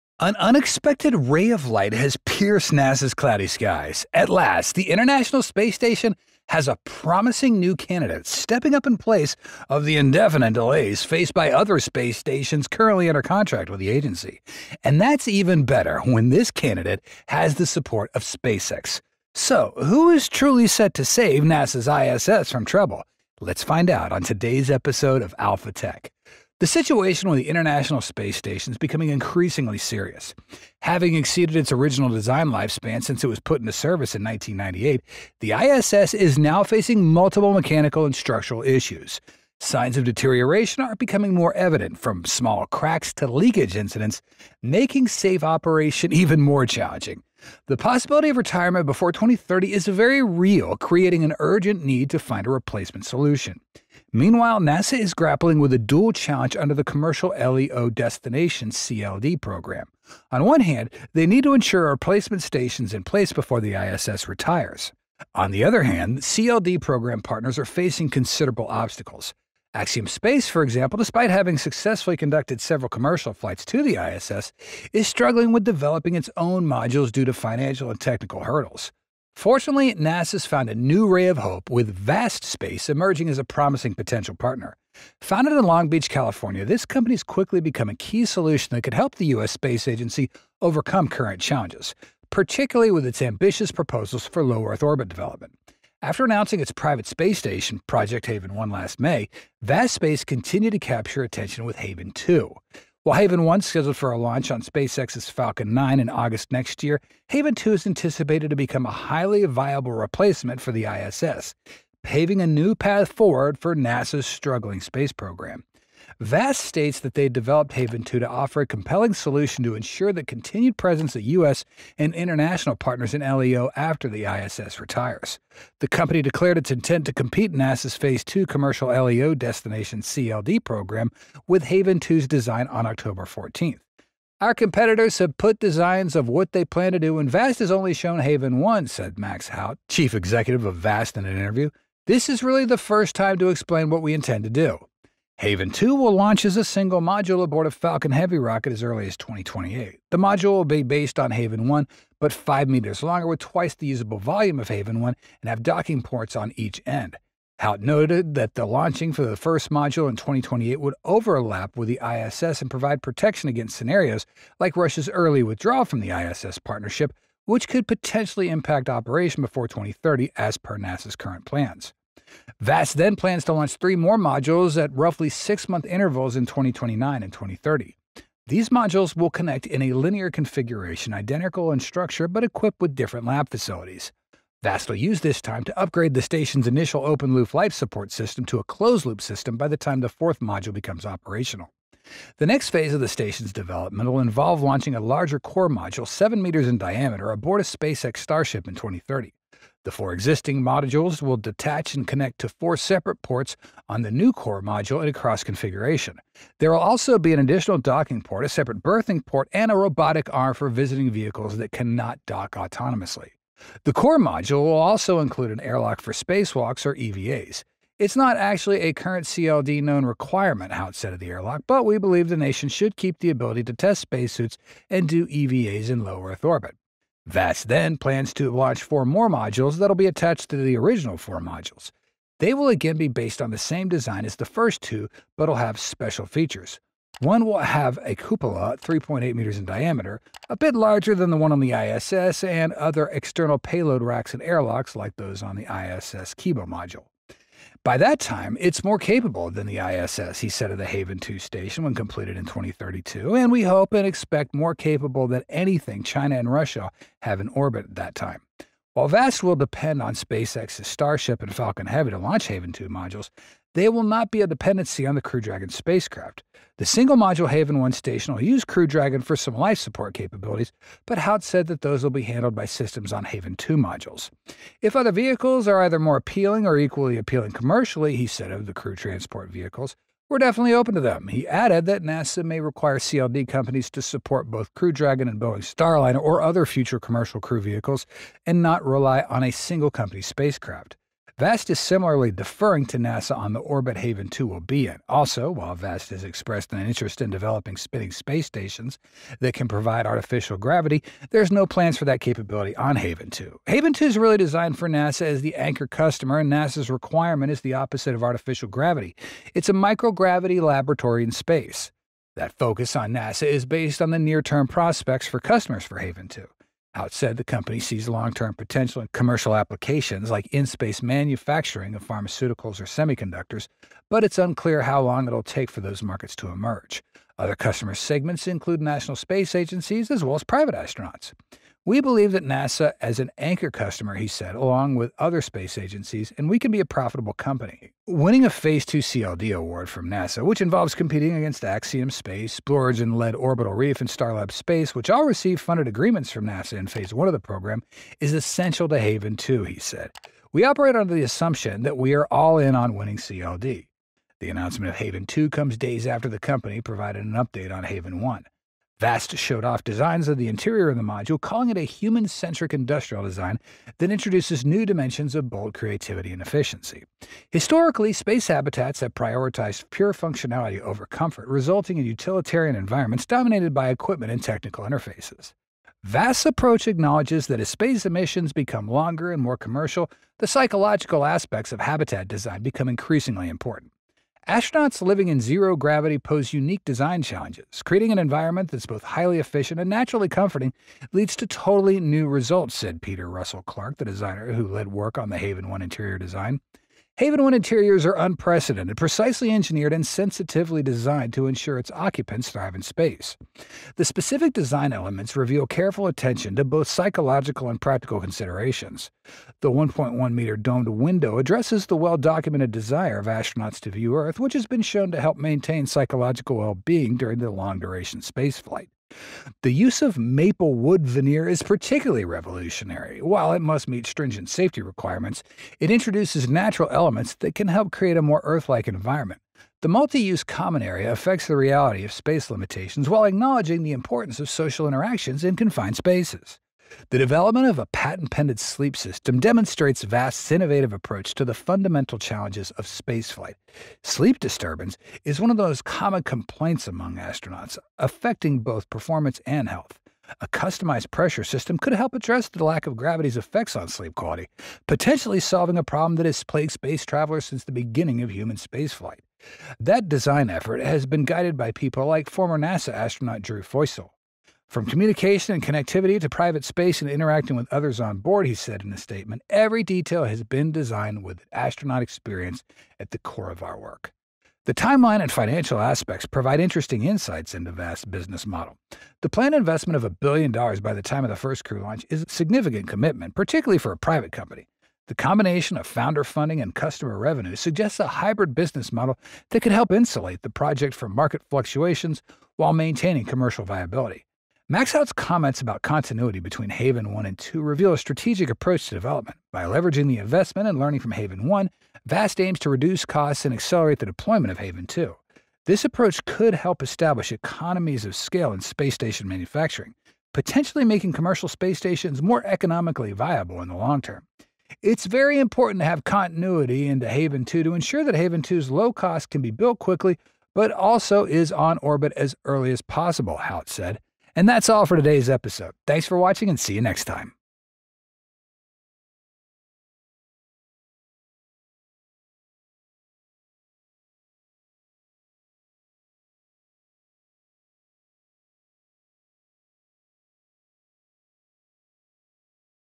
It's Over! SpaceX Big Solution NEW Space Station to solve NASA's BIG TROUBLE...
An unexpected ray of light has pierced NASA’s cloudy skies! At last, the International Space Station has a promising new candidate, stepping up in place of the indefinite delays faced by other space stations currently under contract with the agency.
And that's even better when this candidate has the support of SpaceX.
So, who is truly set to save NASA’s ISS from trouble?
Let’s find out on today’s episode of Alpha Tech:
The situation with the International Space Station (ISS) is becoming increasingly serious. Having exceeded its original design lifespan since it was put into service in 1998, the ISS is now facing multiple mechanical and structural issues. Signs of deterioration are becoming more evident, from small cracks to leakage incidents, making safe operation more challenging. The possibility of retirement before 2030 is very real, creating an urgent need to find a replacement solution.
Meanwhile, NASA is grappling with a dual challenge under the Commercial LEO Destinations (CLD) program. On one hand, they need to ensure a replacement station is in place before the ISS retires. On the other, CLD program partners are facing considerable obstacles. Axiom Space, for example, despite having successfully conducted several commercial flights to the ISS, is struggling with developing its own modules due to financial and technical hurdles.

Fortunately, NASA has found a new ray of hope with Vast Space emerging as a promising potential partner. Founded in Long Beach, California, this company has quickly become a key solution that could help the U.S. space agency overcome current challenges, particularly with its ambitious proposals for low Earth orbit development.
After announcing its private space station project Haven-1 last May, Vast Space continued to capture attention with its Haven-2 project. While Haven-1 is scheduled for launch on a SpaceX Falcon 9 rocket in August 2025, Haven-2 is anticipated to become a highly viable replacement for the International Space Station (ISS), paving a new path forward for NASA’s struggling space program.
Vast states that they developed Haven-2 to offer a compelling solution to ensure the continued presence of U.S. and international partners in low Earth orbit (LEO) after the ISS retires. The company declared its intent to compete in NASA’s Phase 2 Commercial LEO Destinations (CLD) program with its Haven-2 design on October 14.